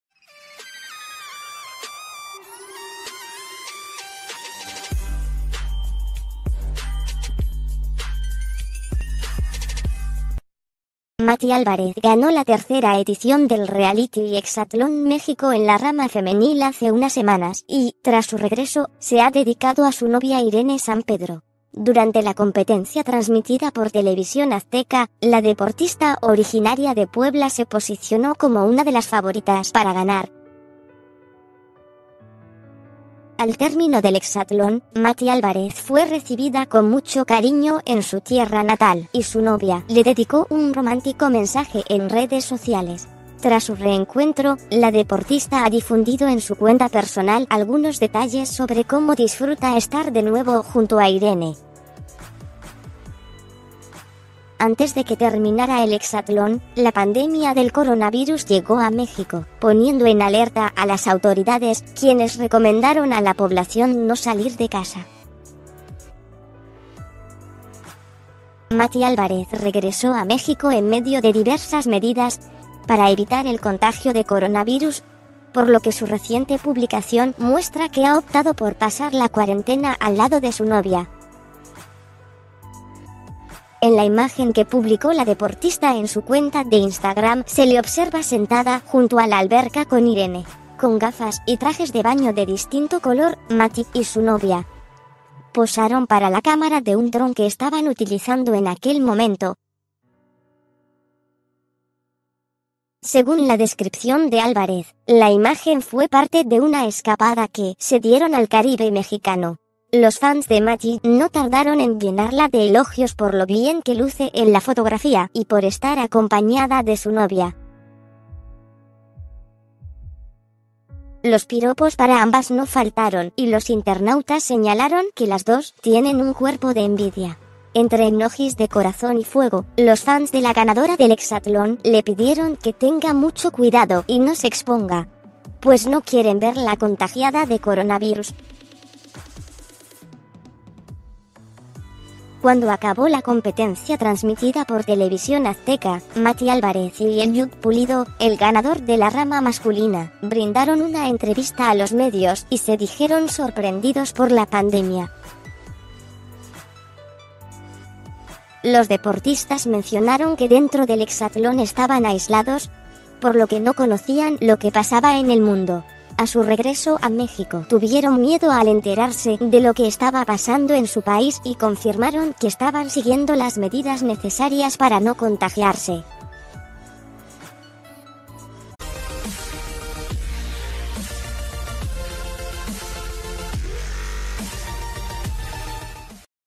Mati de Exatlón muestra desde la alberca cómo pasa la cuarentena con su novia
Mati Álvarez ganó la tercera edición del reality Exatlón México en la rama femenil hace unas semanas y, tras su regreso, se ha dedicado a su novia Irene Sampedro...